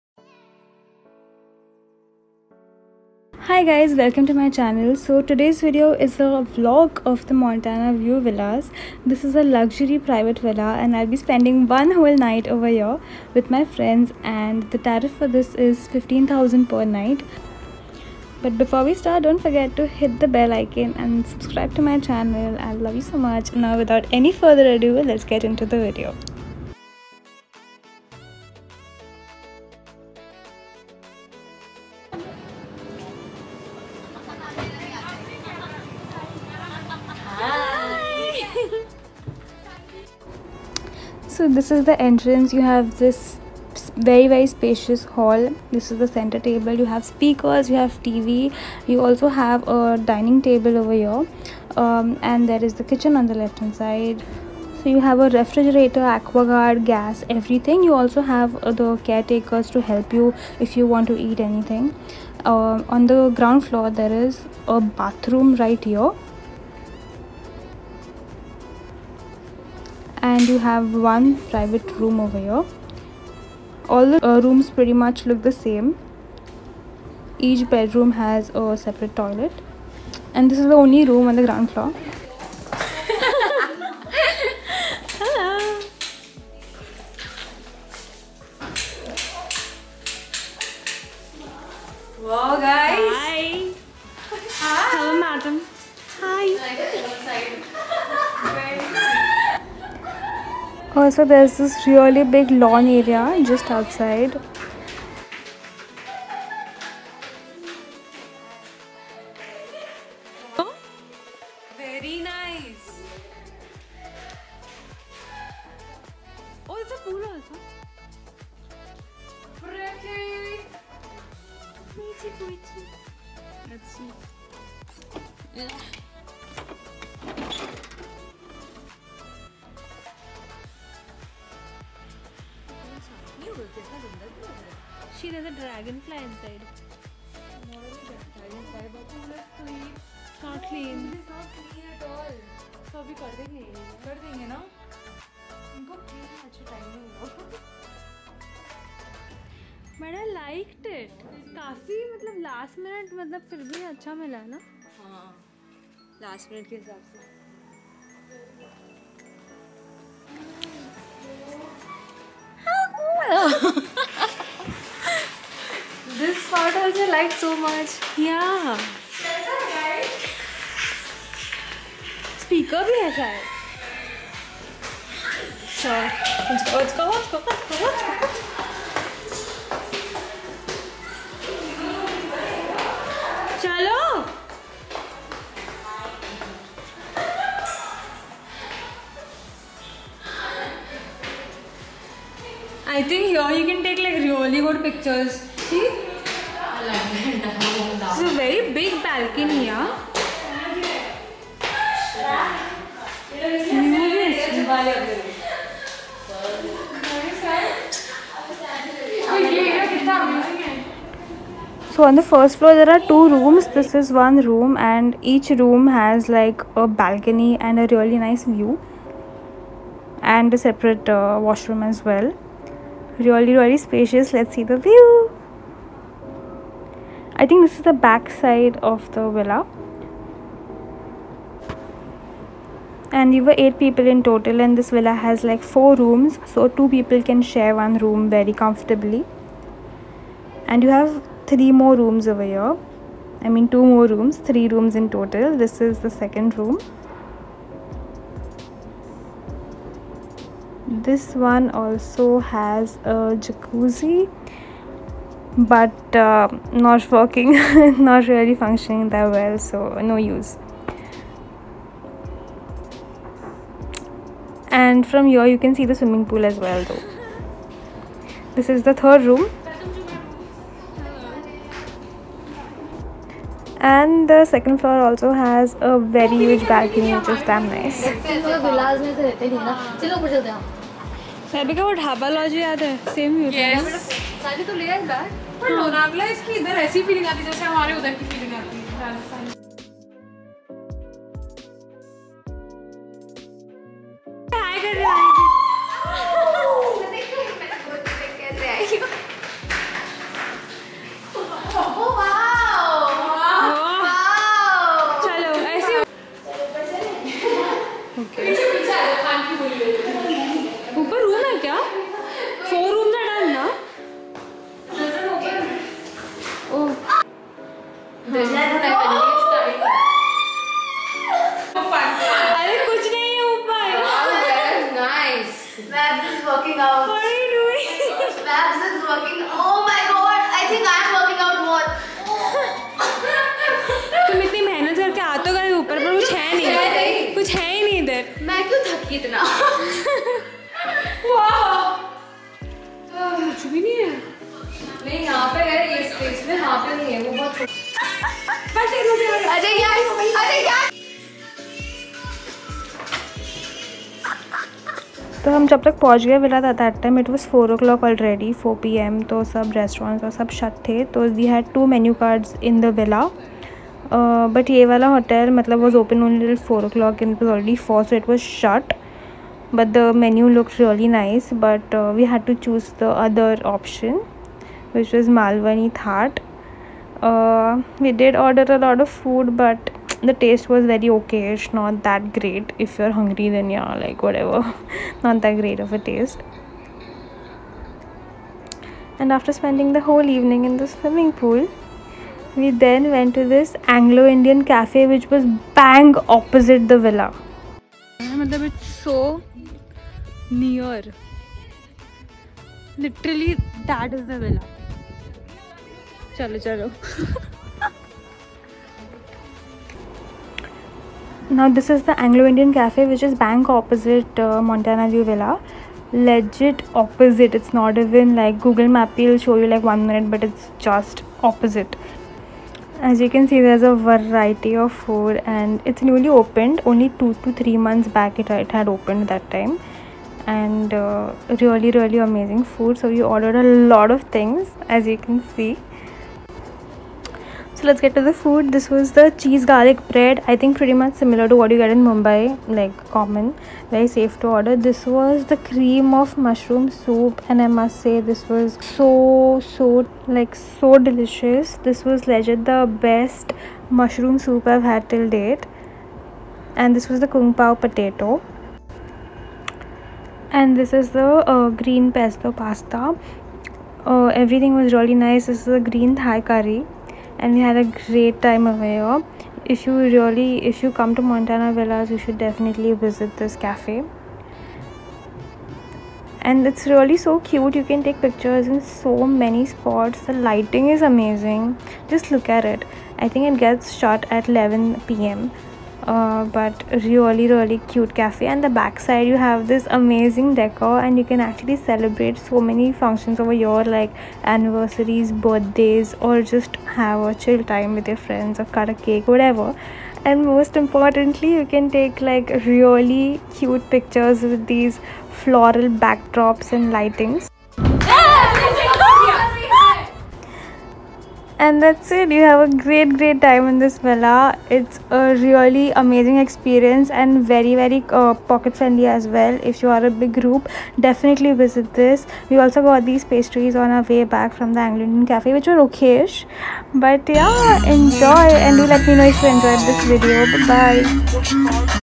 1 NIGHT IN MONTANA VUE - LUXURY VILLA WITH PRIVATE POOL IN LONAVALA FOR RENT - STAYCATION VLOG
Montana vue villas is a luxury villa with a private pool
15000 INR per night

MY EXPERIENCE AT THE LALIT MUMBAI - 5 STAR HOTEL 3BHK ROOM UNDER RS 2500/- || VLOG 2021